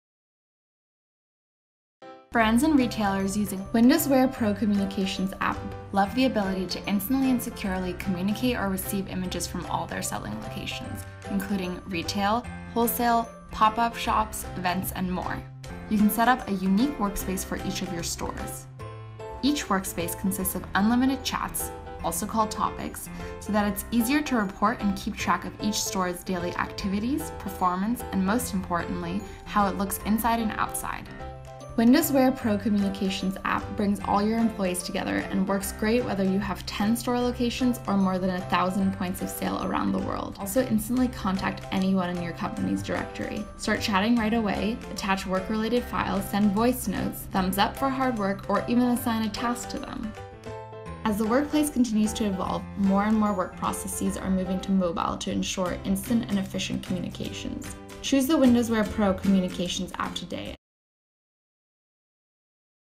WindowsWear PRO Real-Time Retail Communications App!
WindowsWear's Real-Time Retail Communications App
allows your team to instantly and securely receive real-time communications, images and
attachments from all your selling locations worldwide including
retail, wholesale, pop-up shops, events, and more.  It helps you
answer the question "what do all of my stores & other selling channels look like now?"  Senior retail professionals who manage
teams can also send one-way broadcasts and send directives
effortlessly (think of our app like WhatsApp meets Facebook Messenger meets Pinterest).  The App works on any device (desktop, mobile, web), and you will not have to dig through emails anymore to find or request for images and to communicate with all your stores.